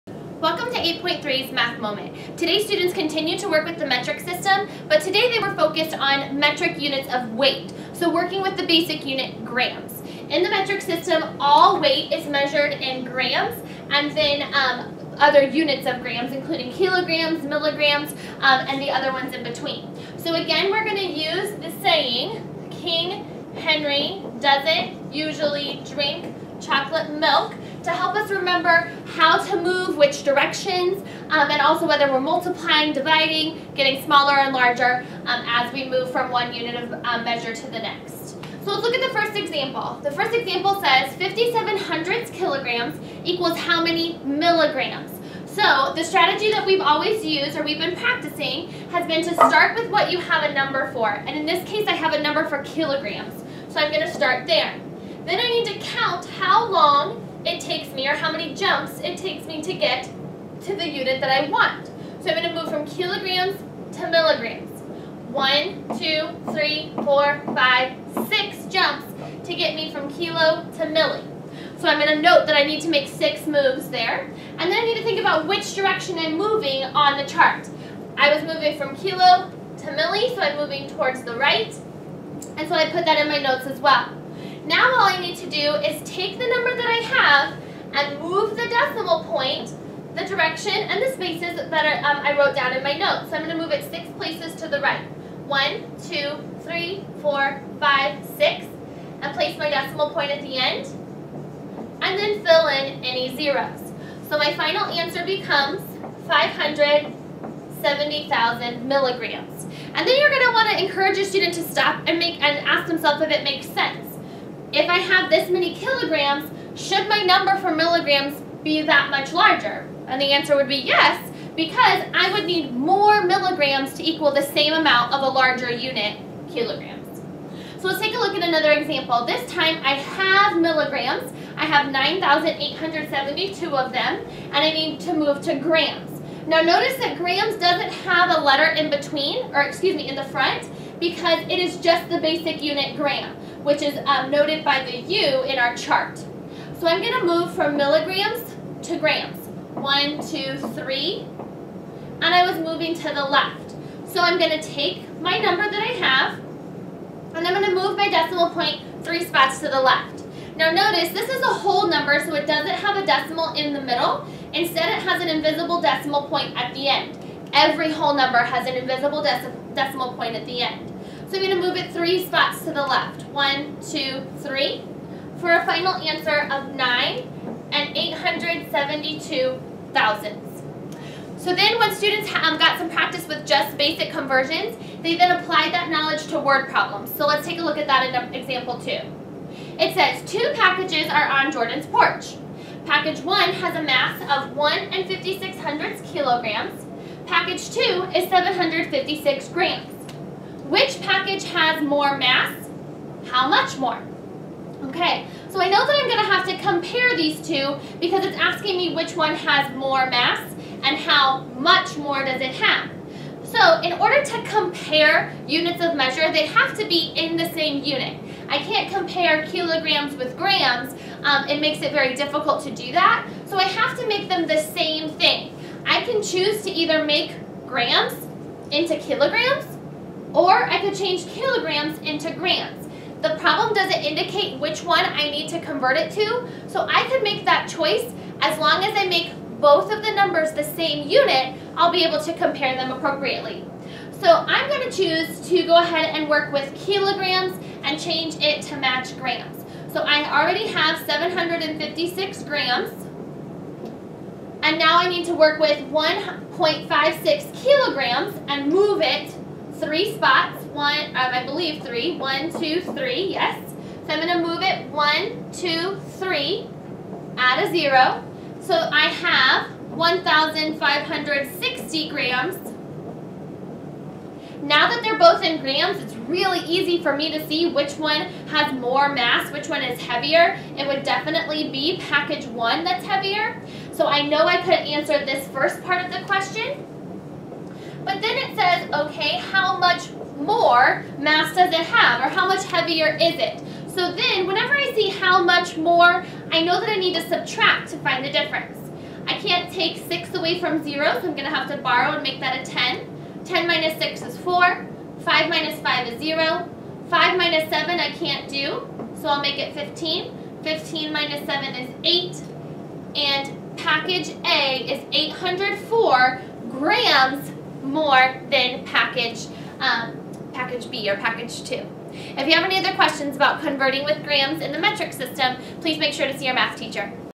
5th Grade 8-03
Metric Units of Mass
Students learned how to convert among metric units of mass.